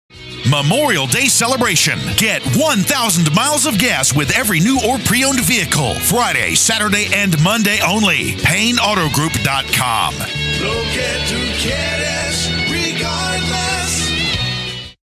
Memorial Day Celebration | Payne Rio Grande City Ford | Rio Grande City, Texas
Payne Rio Grande City Ford invites you to visit us during the Memorial Day Celebration during May 27, 2022 through May 30, 2022! THREE DAYS ONLY!

Get 1,000 Miles of Gas when you purchase any new or used car, truck or SUV! Start shopping!